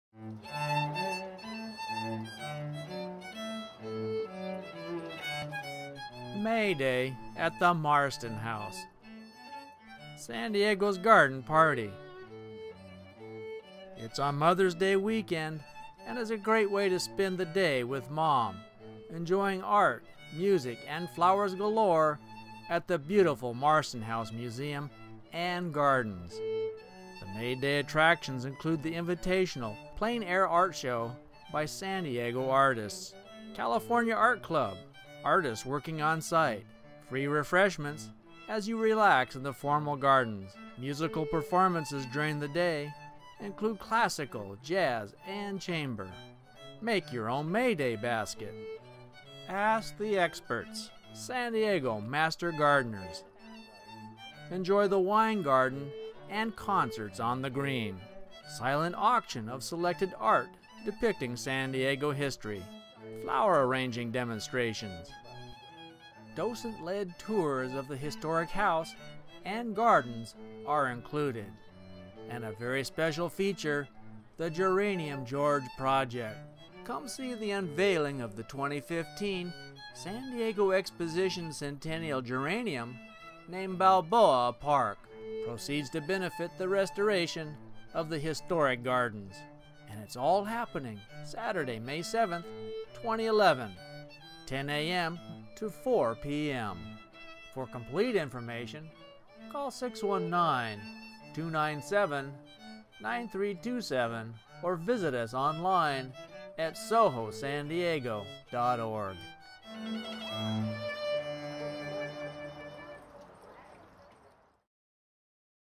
May Day at The Marston House
My video promoting May Day at the Marston House Museum and Gardens